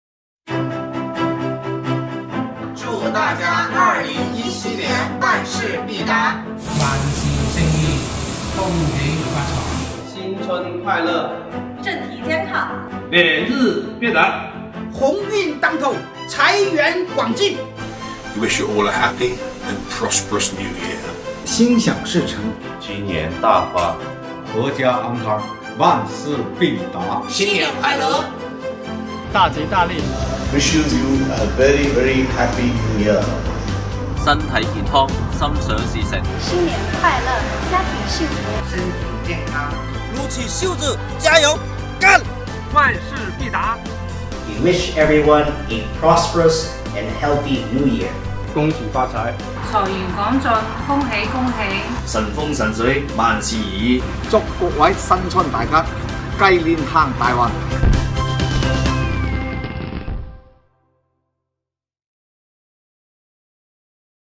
Happy Chinese New Year 2017
Chinese New Year Greetings from team of Be-Tech, the year of the Rooster.